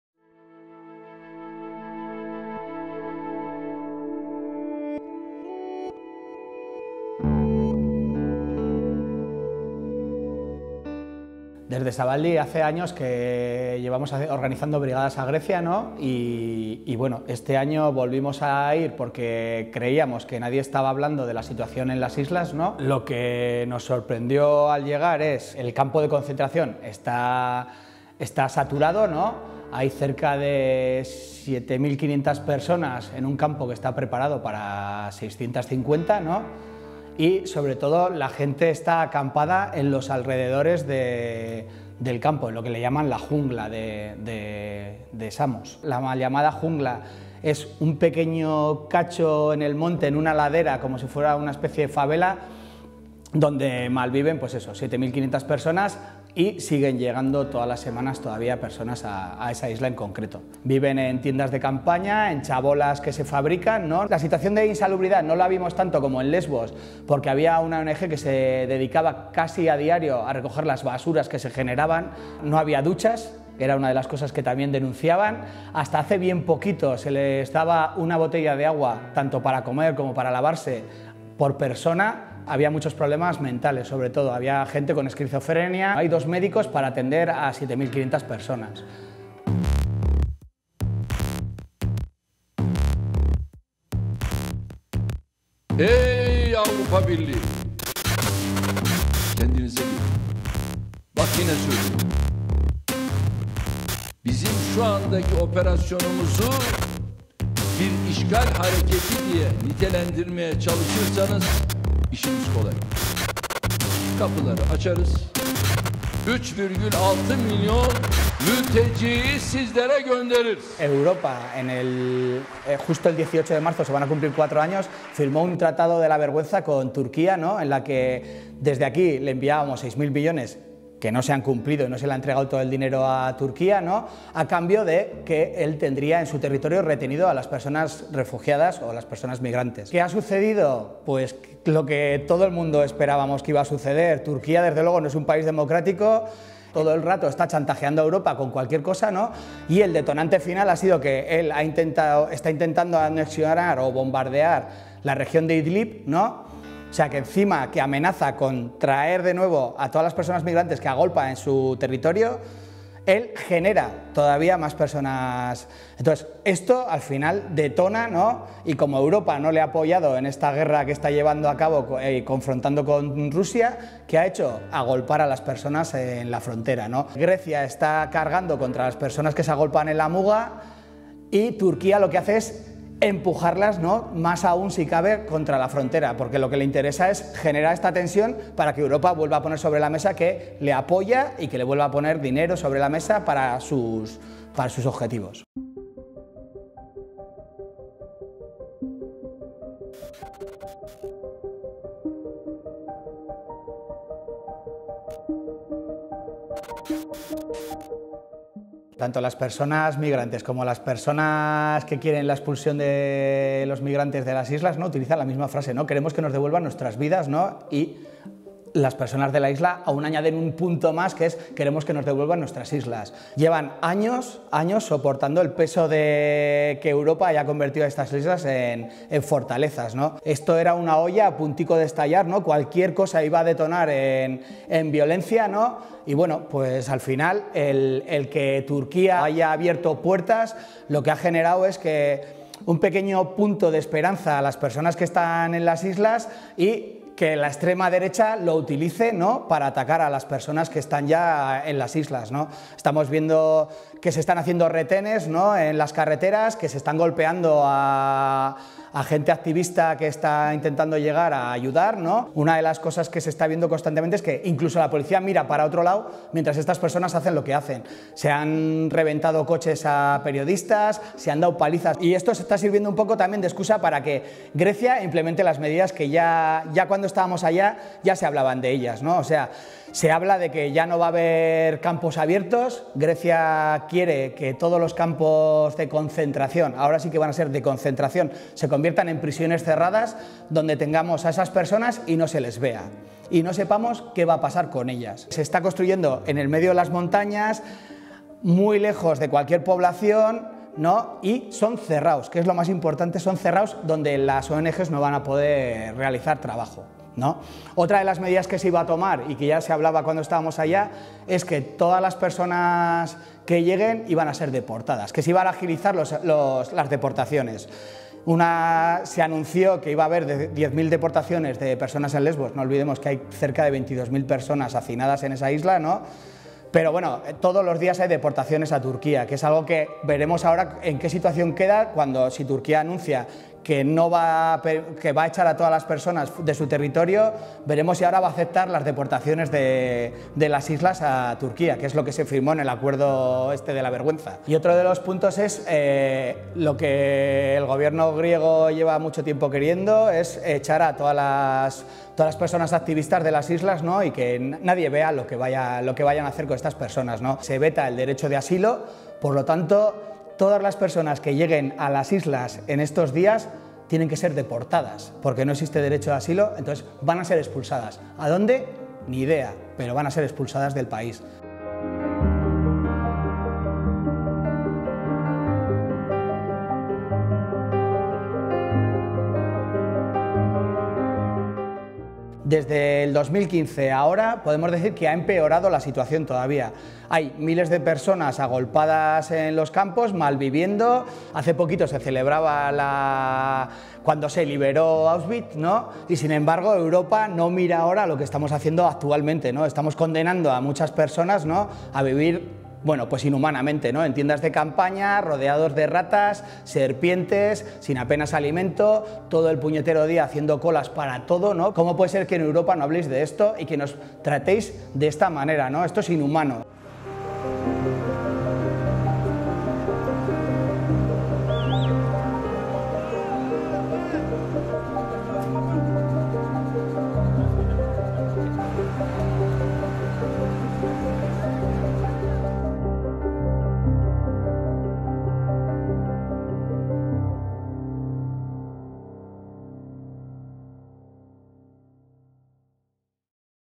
La crisis humanitaria en la frontera entre Grecia y Turquía
David Sanchez, fotografo de Ekinklik e internacionalista miembro de Zabaldi, ha estado recientemente en Grecia y ha sido testigo de la situación de emergencia humanitaria que se vive en las islas griegas. Esta entrevista cobra mayor valor ahora que las manifestaciones convocadas para este fin de semana en solidaridad con las personas migrantes han sido desconvocadas por el coronavirus.